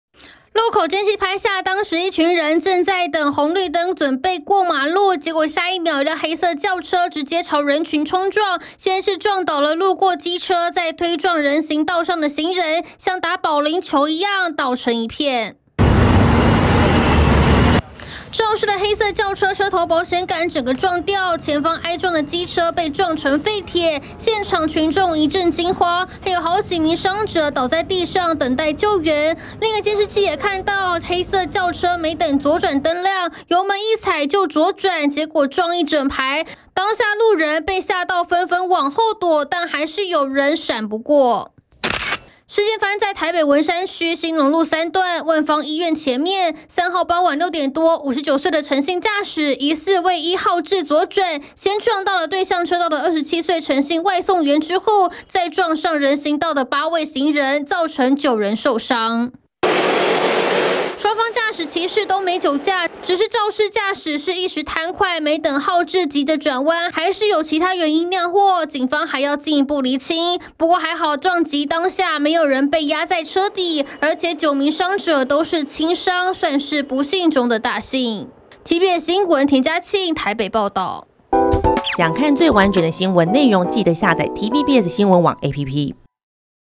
急啥? 駕駛闖燈左轉 撞機車再撞行人釀9傷｜TVBS新聞
台北文山區萬芳醫院前 晚間發生一起車禍意外，撞擊瞬間曝光，當時駕駛闖紅燈左轉，結果先撞到對向路過的外送員後，又再推撞波及一旁等待過馬路的行人，車頭撞爛之外，也造成9人受傷。